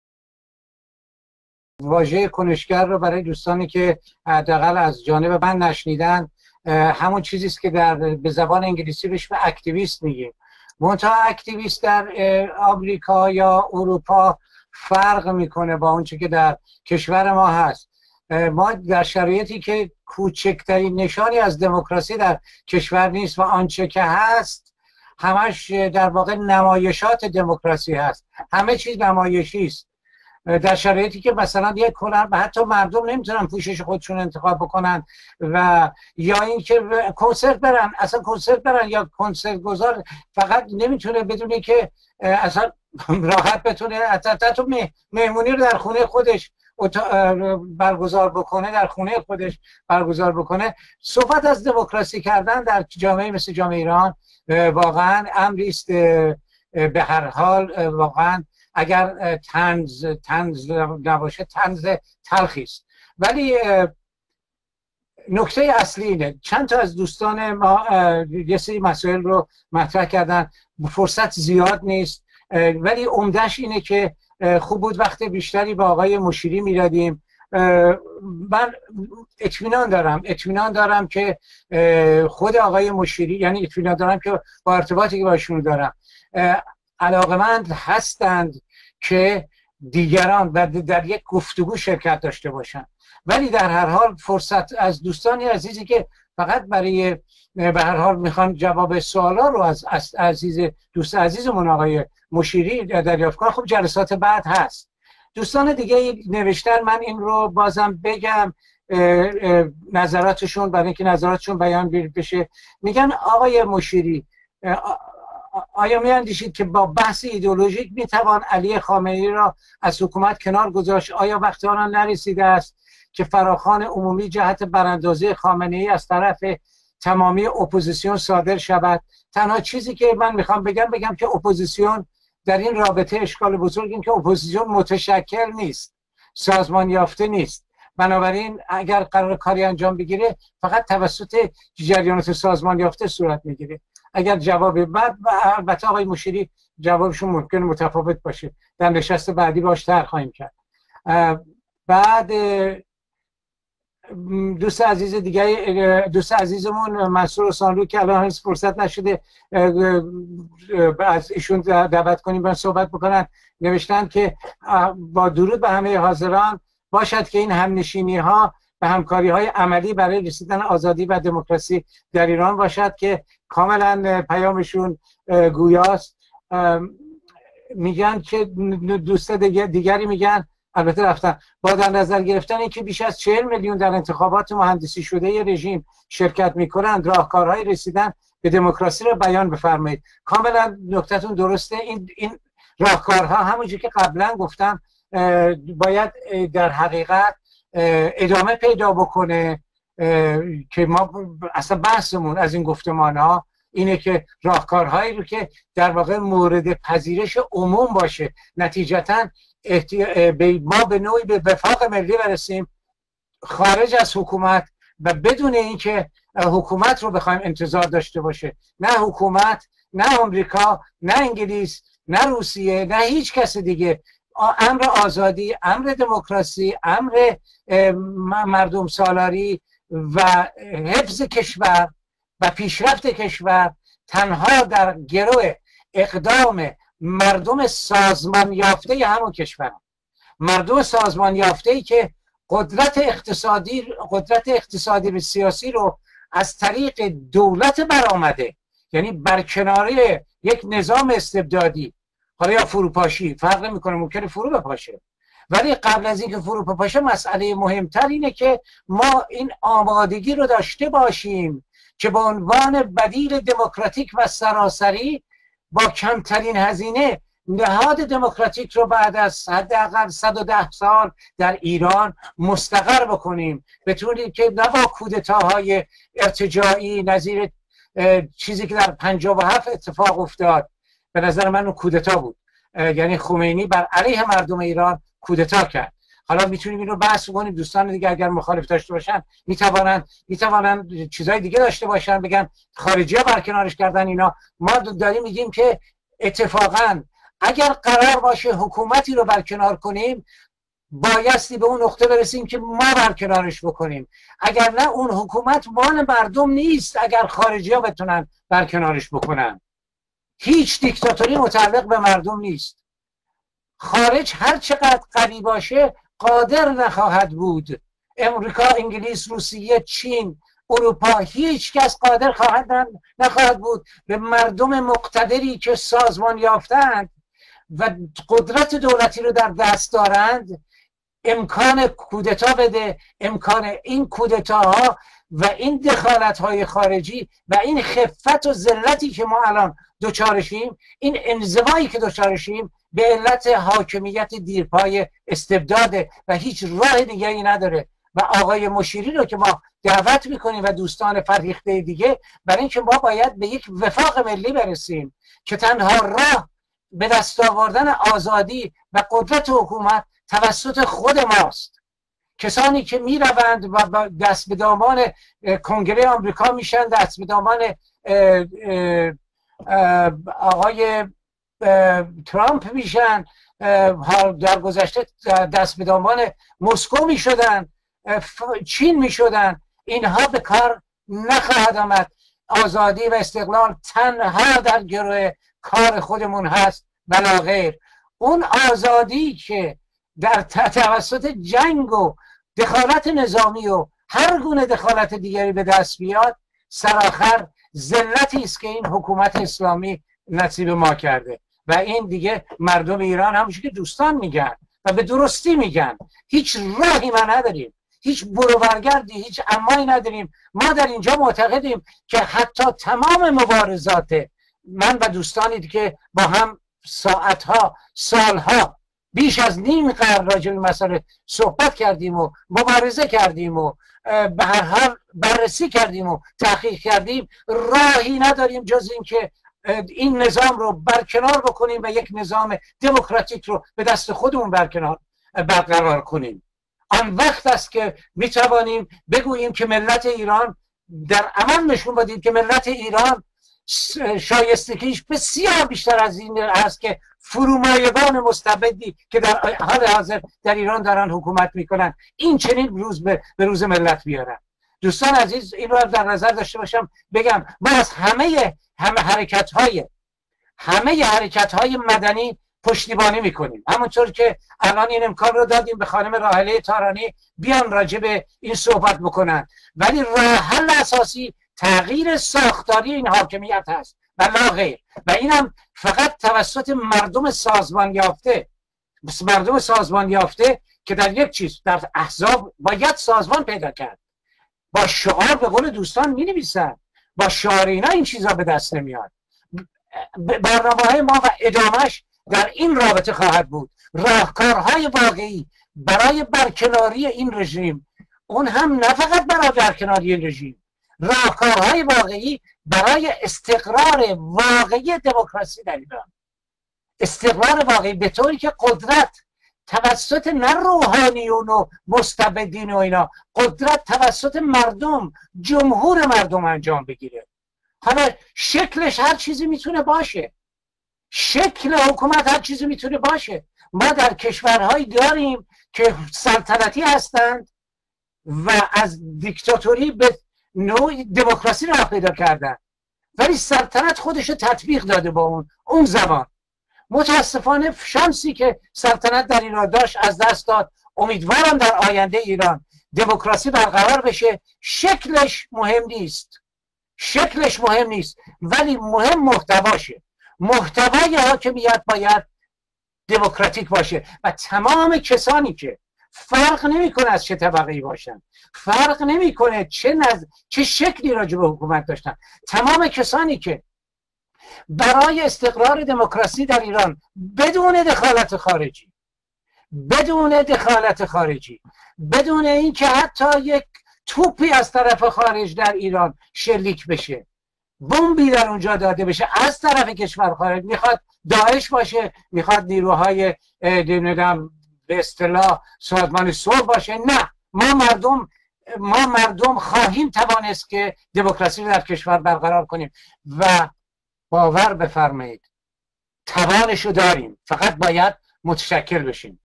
رضا بدیعی در نشست وبیناری گذار؛ کنشگران و گذار به دموکراسی در ایران
رضا بدیعی در نشست وبیناری گذار از حاکمیت اسلامی درباره مفهوم کنشگر و نقش آن در گذار به دموکراسی در ایران، و همچنین به مسائل مربوط به دموکراسی در این کشور پرداخت.
به گزارش گذار، این کنشگر سیاسی و فعال حقوق بشر، با اشاره به مسائل مربوط به گذار به دموکراسی در ایران در گذشته و حال، به مفهوم کنشگری به قصد تغییر و اثرگذاری در ساختار سیاسی حاکم بر ایران و پیش بردن پروسه گذار به دموکراسی پرداخت.
رضا بدیعی از جمله کنشگران سیاسی باسابقه است که بیش از نیم قرن سابقه فعالیت و مبارزه سیاسی پیش از انقلاب 57 و پس از آن را تا به امروز در کارنامه خود دارد.
این کنشگر ایرانی ساکن آمریکا در جریان نشست وبیناری گذار از حاکمیت اسلامی به دموکراسی، همچنین درباره مفهوم گذار به دموکراسی و شرایط آن و ویژگی‌های کنشگران در این پروسه پرداخت.
نشست‌های وبیناری گذار از حاکمیت اسلامی به دموکراسی در دومین یک‌شنبه هر ماه به صورت اینترنتی برگزار می‌شود و جمعی از کنشگران سیاسی و مدنی و مدافعان حقوق بشر ایرانی ضمن تبادل نظر و همفکری، درباره هماهنگی کنش‌ها و یافتن راهکارهای استقرار دموکراسی در ایران در این نشست‌ها همراهی می‌کنند.
در ادامه فایل صوتی و تصویری مربوط به سخنان رضا بدیعی در جریان نشست وبیناری گذار در روز یک‌شنبه 13 آگست 2017 (22 مردادماه 1396) آمده است.